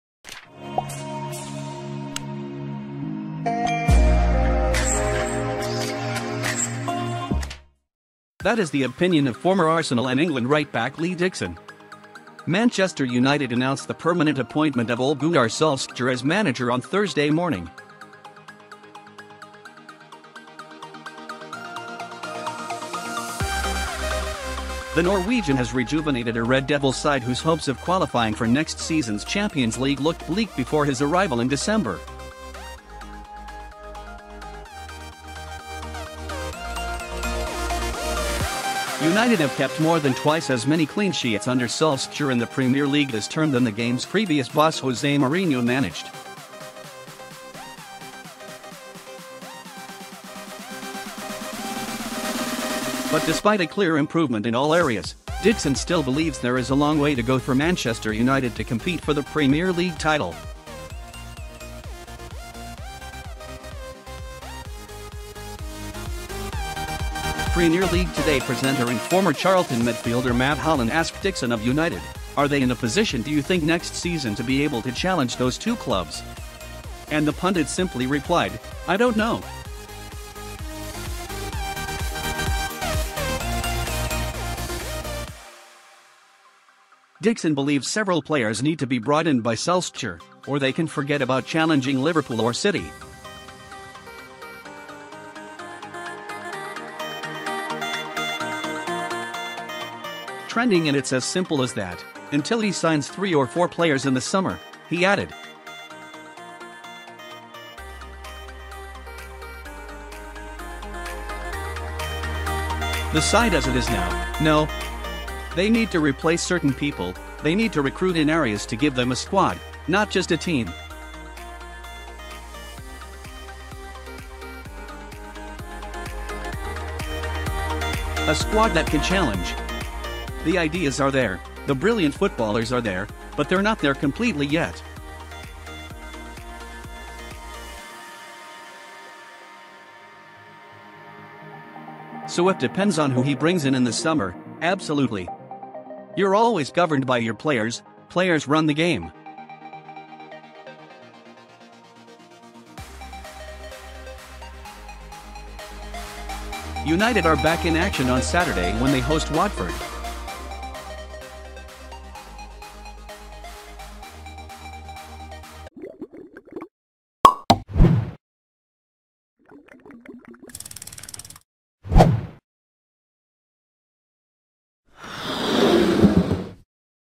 Can Man Utd topple Liverpool and City next season? Dixon gives three-word response Man City News
That is the opinion of former Arsenal and England right-back Lee Dixon. Manchester United announced the permanent appointment of Ole Gunnar Solskjaer as manager on Thursday morning. The Norwegian has rejuvenated a Red Devils side whose hopes of qualifying for next season’s Champions League looked bleak before his arrival in December. United have kept more than twice as many clean sheets under Solskjaer in the Premier League this term than the games previous boss Jose Mourinho managed. But despite a clear improvement in all areas, Dixon still believes there is a long way to go for Manchester United to compete for the Premier League title. Premier League Today presenter and former Charlton midfielder Matt Holland asked Dixon of United: “Are they in a position do you think next season to be able to challenge those two clubs?” And the pundit simply replied: ”I don’t know.” Dixon believes several players need to be brought in by Solskjaer, or they can forget about challenging Liverpool or City. Trending    “And it’s as simple as that, until he signs three or four players in the summer," he added. “The side as it is now, no. “They need to replace certain people, they need to recruit in areas to give them a squad, not just a team. “A squad that can challenge. “The ideas are there, the brilliant footballers are there, but they’re not there [completely] yet. “So it depends on who he brings in in the summer, absolutely. “You’re always governed by your players, players run the game.” United are back in action on Saturday when they host Watford.